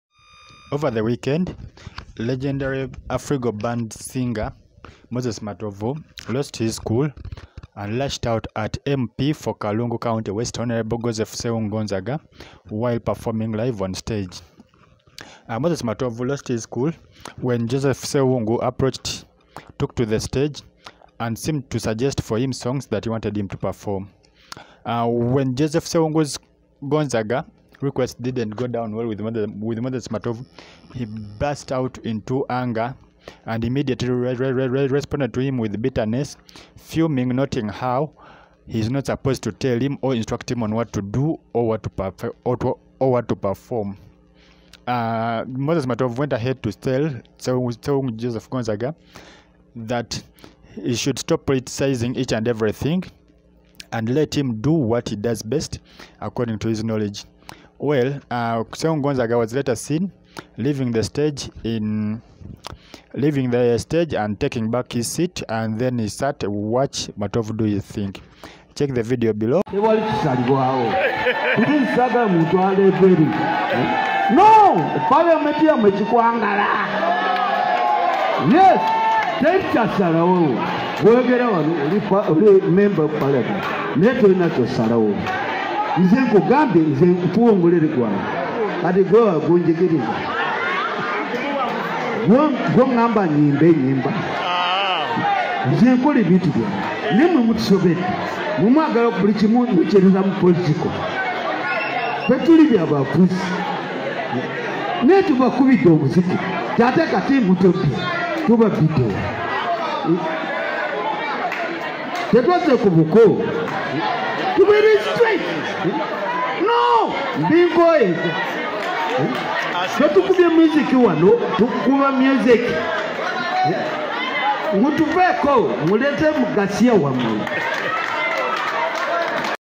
Parliament yamwe kikwangala - Moses Matovu blasts Hon. Joseph Ssewungu Gonzaga
Afrigo band veteran musician Moses Matovu lashed out at MP for Kalungu County West Hon. Joseph Ssewungu Gonzaga while performing on stage.
Moses Matovu blasted Hon. Joseph Ssewungu after he took to the stage and seemed to suggest songs that he wanted him to perform.

Hon. Joseph Ssewung’s request didn’t go down well with Moses Matovu and he immediately responded with bitterness fuming as he noted that he be let do what he does best than instructing him.